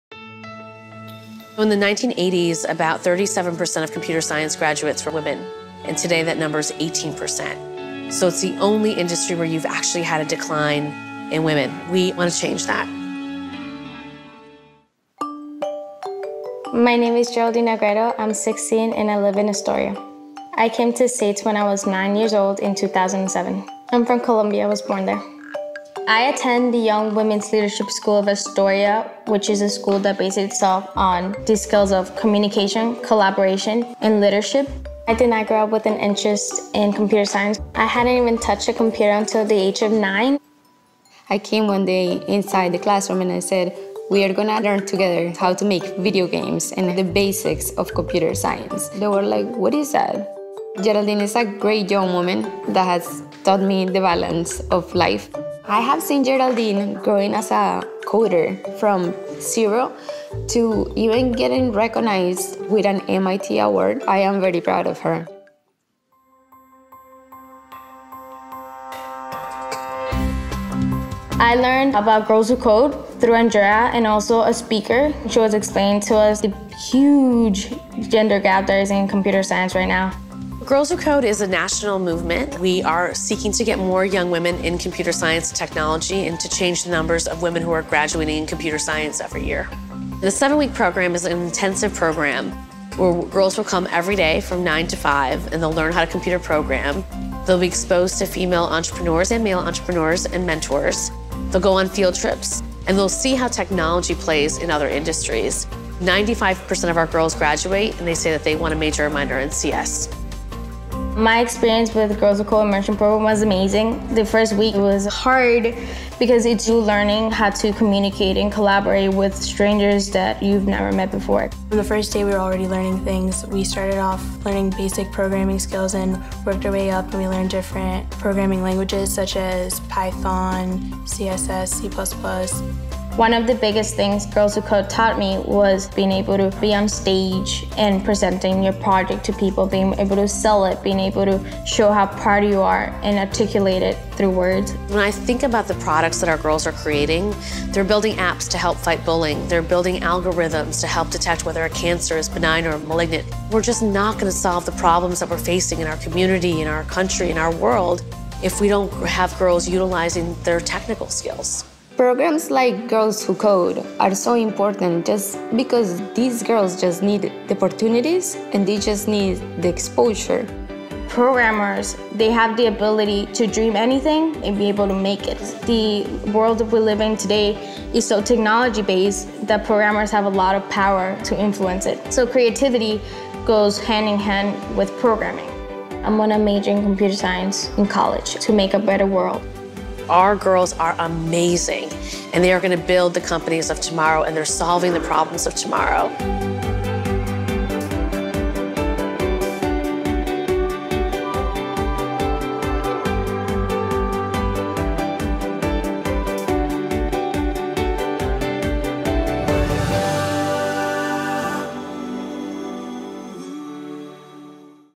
Adobe’s Youth Coding Initiative: Girls Who Code
16 year old Geraldine Agredo came to New York from Colombia when she was nine. A student at the Young Women’s Leadership School in Astoria, a neighborhood in Queens, she didn’t touch a computer until she came to the United States. Geraldine was inspired to program by a speaker who came to her school, and got involved with Girls Who Code through her teacher. In 2014, she and her coding classmates won an MIT Club of New York award for their recycling game, Green Code. Geraldine believes programming and creativity go hand in hand, and is excited to major in computer science to make a better world. Girls Who Code is a partner of Adobe’s Youth Coding Initiative, an effort to bring computer science education to girls and under-represented youth.